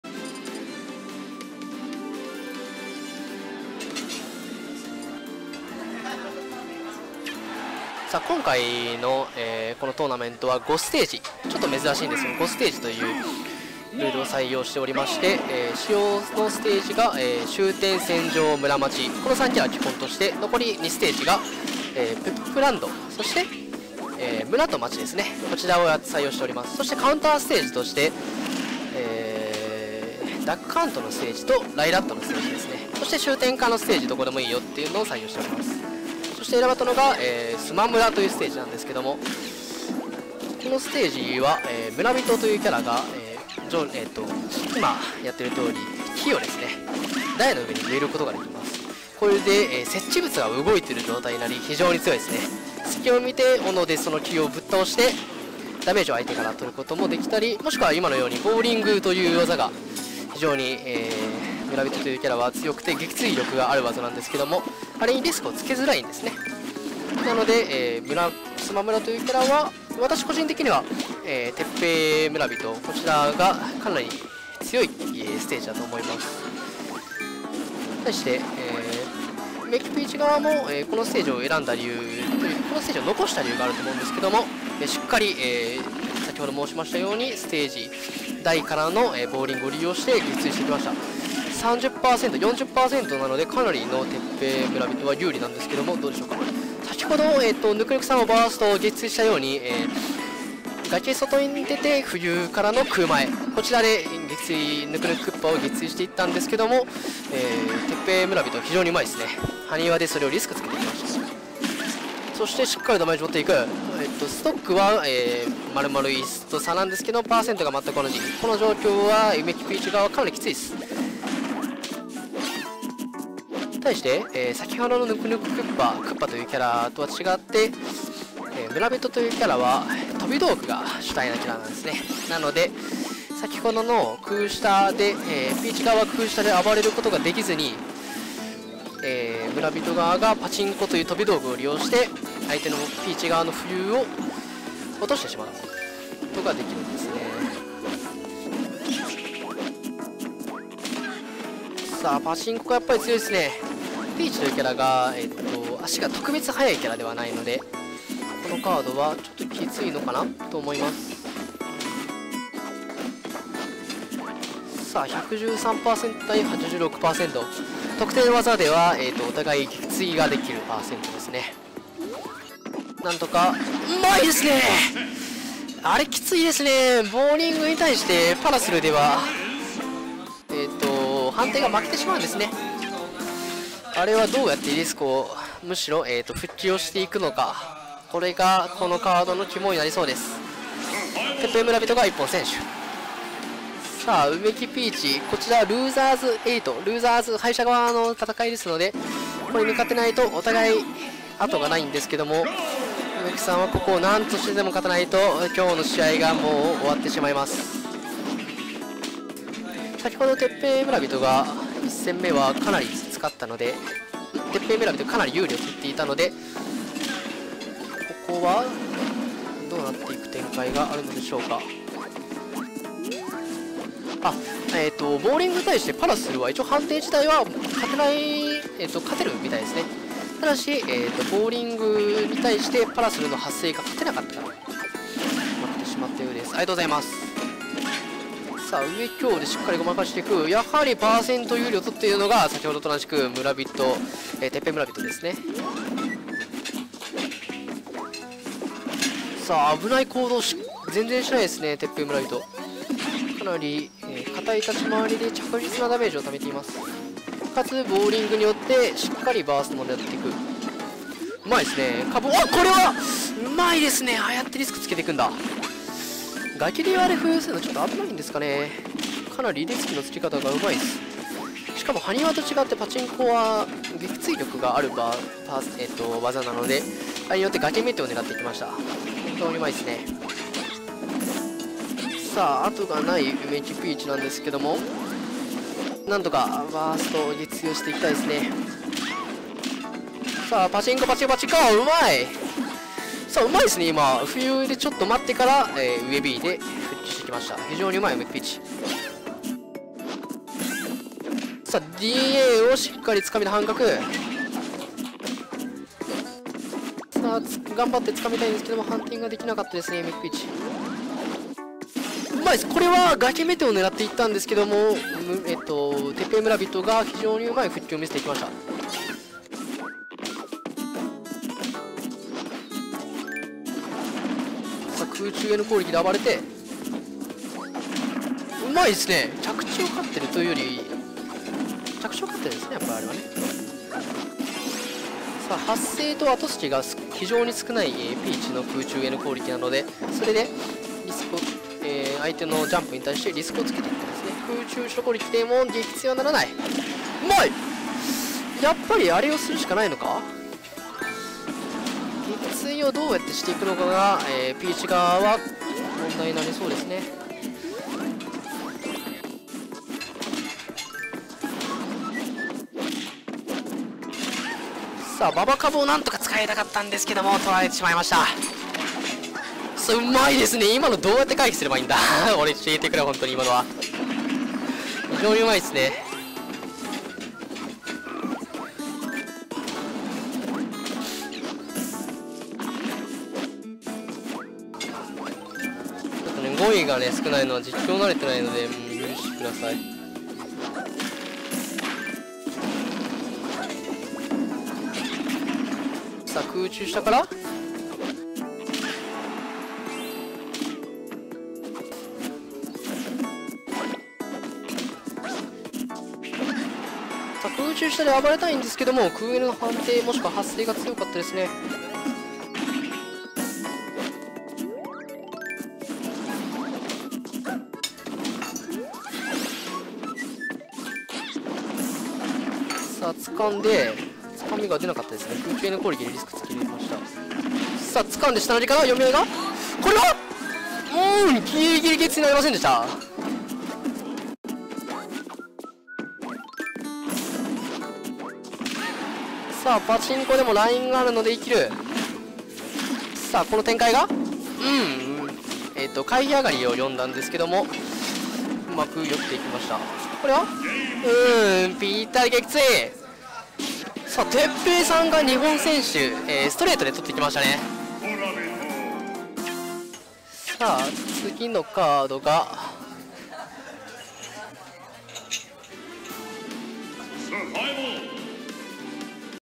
eSports Runner杯 LB8 Teppei vs Umeki/eSports Runner Tournaments
eSports Runner Tournaments : 2016 Mar.26th @ Pasera, Tokyo

Losers Bracket8 : てっぺい/Teppei(Villager) vs うめき/Umeki(Peach).

2stock, double elimination / 2ストック、ダブルエリミネーション.